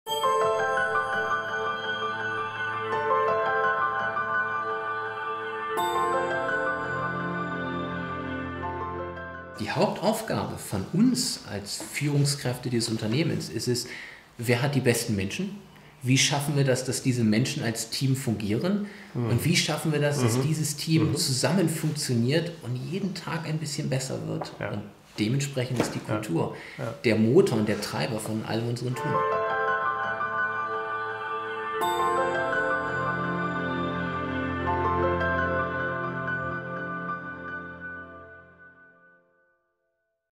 Der Stellenwert von Unternehmenskultur
Welchen Stellenwert hat Unternehmenskultur? Oliver Bruns, CEO von NEVEON, gibt uns seine Ansichten.